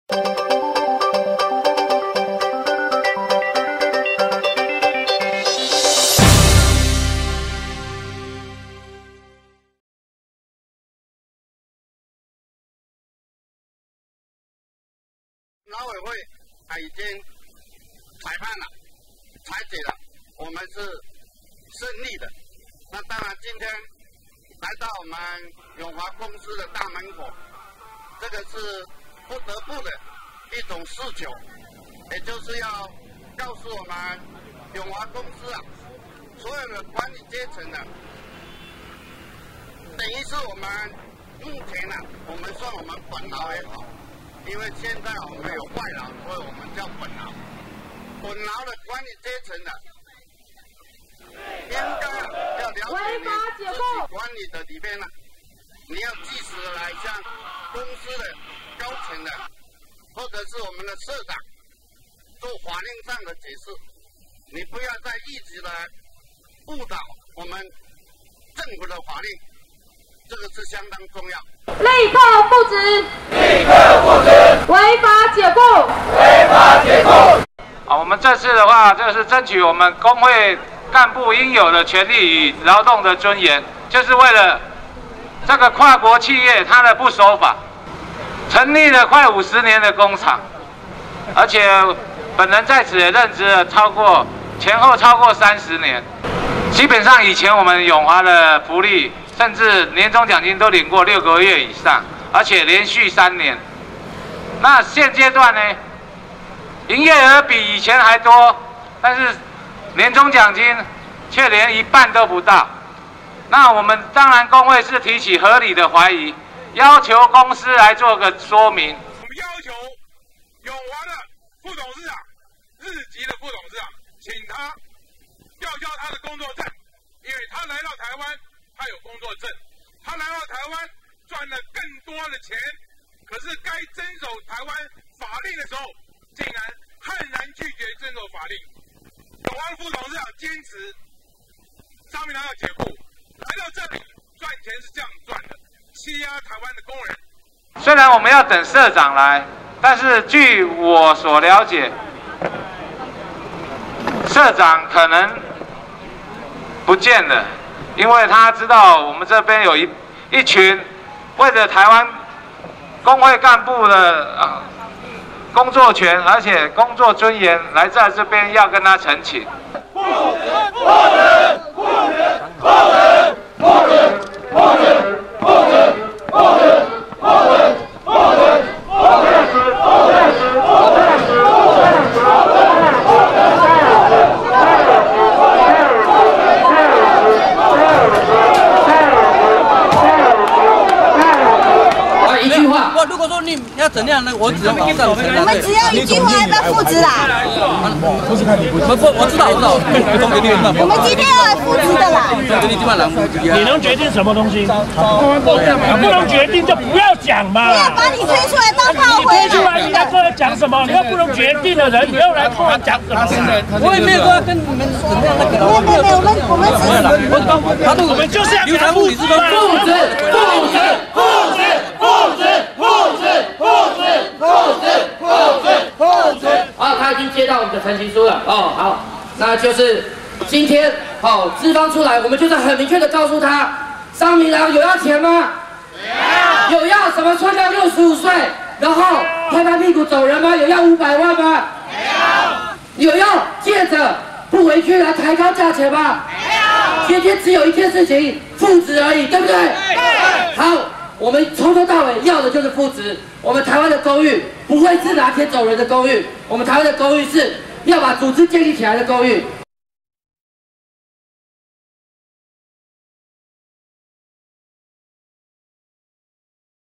20120518「政府無能，勞工自救-踐踏台灣法令，百人包圍永華機械公司」
桃園永華機械公司，所屬永華機械工會的常務理事商明郎跟公司談年終獎金的時，卻因為發放文宣指出公司有賺錢，而遭到公司開除。
商明郎向勞委會申請裁決結果確認永華公司101年1月6日解雇商明郎常務理事行為無效。多次向永華公司要求復職確未得到正面回應。
因此5月18日 桃園縣產業總工會與其他多個工會團體，聚集在永華機械公司門口，企圖阻撓公司日籍幹部進入公司參加董事會議，現場上百民聲援團體與民眾；一度衝破永華機械公司的大門；要求擁有決策權的幹部出面回應，但至當日中午還是尚未得到永華公司的回應。